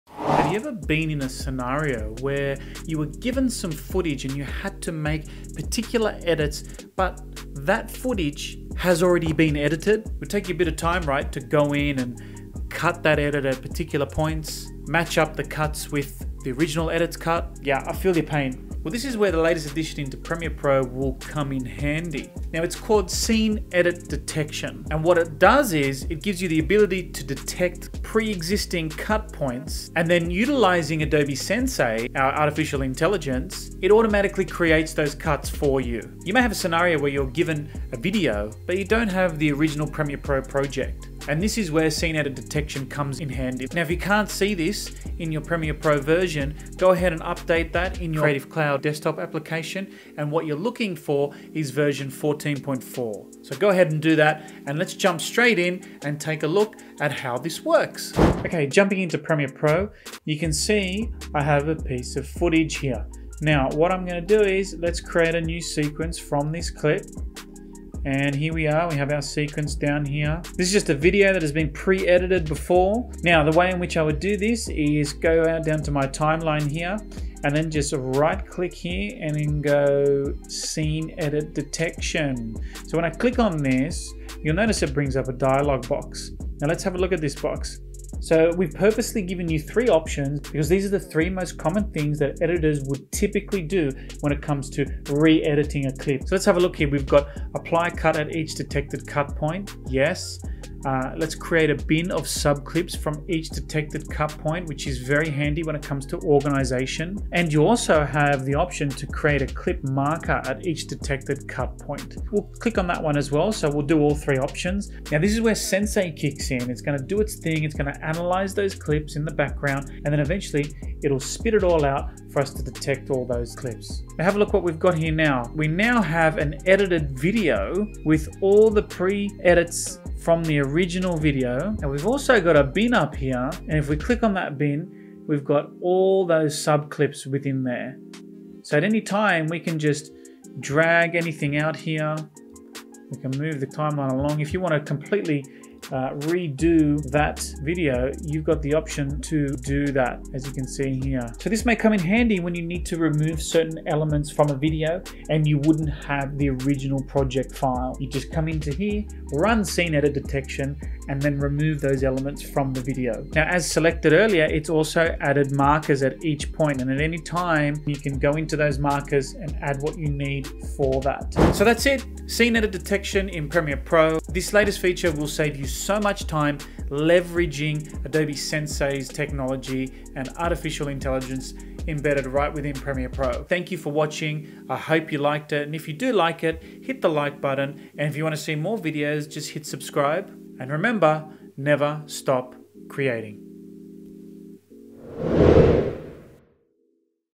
Editing existing footage using Scene Edit Detection in Adobe Premiere Pro
Here's a scenario that I'm sure every video editor has been faced with once or twice... ever been handed a video that has already been edited and you don't have access to the existing Premiere Pro Project?

Well, don't waste hours making manual edits when you can use Scene Edit Detection in Premiere Pro, I know this will save you hours. Let me know what you think. Enjoy!